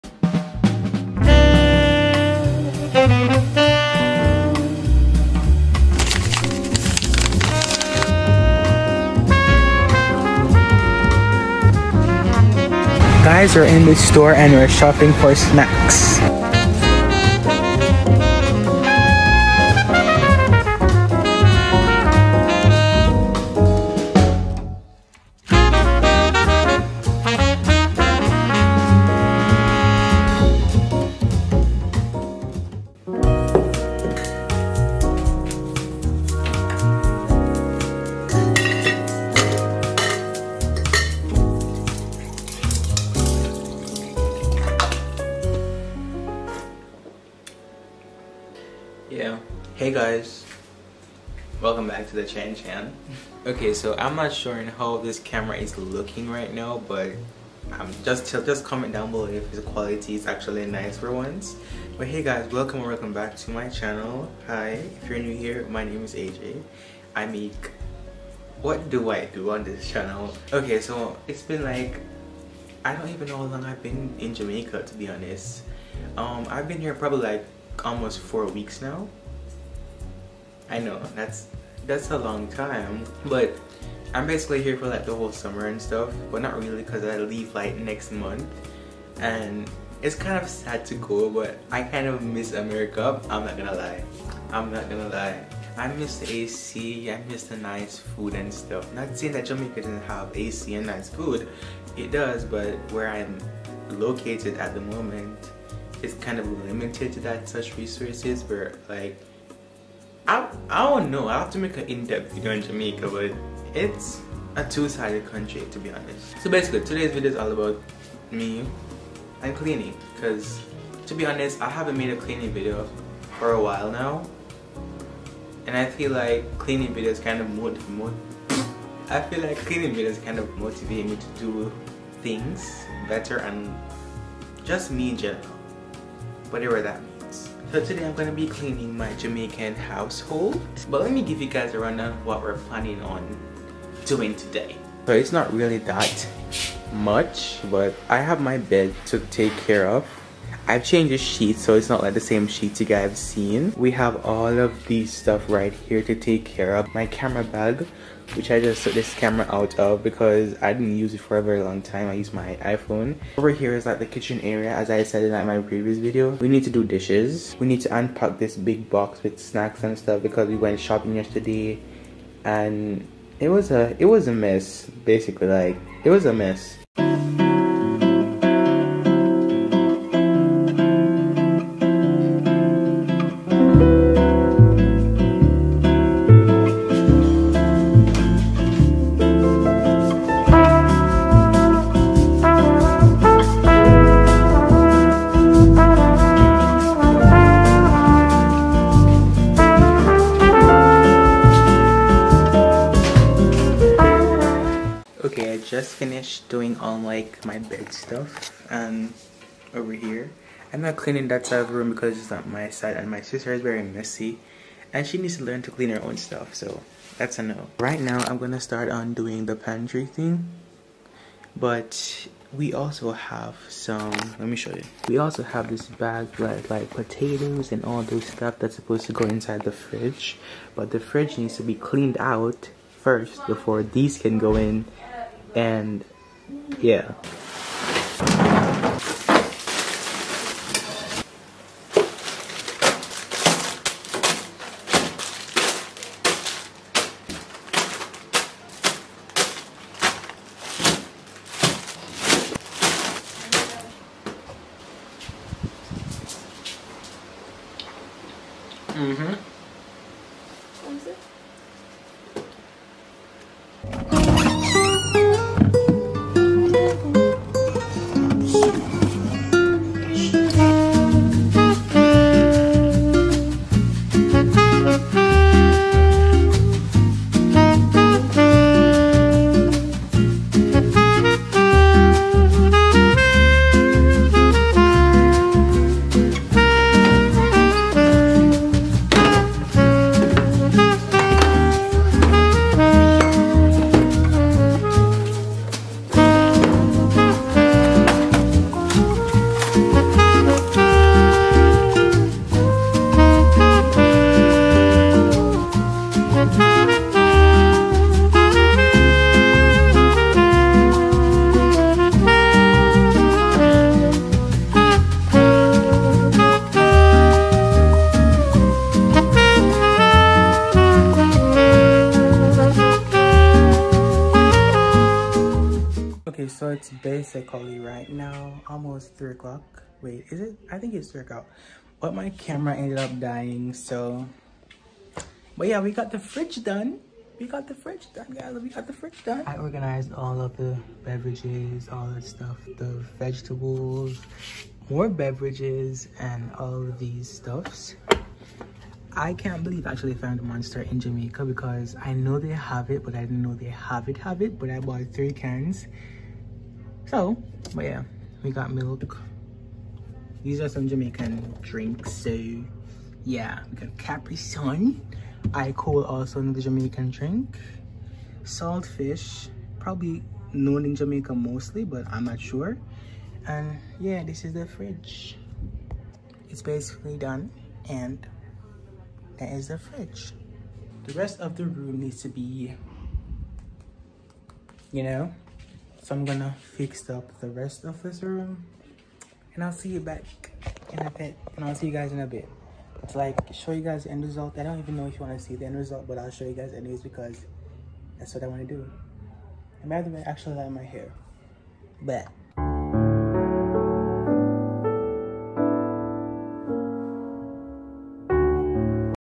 Jamaica diaries | cleaning + organizing my Jamaican home, grocery shopping etc
FAQ ★

 17
2/3/06
Camera - Canon eos 250d / iPhone 11
Aquarius ♒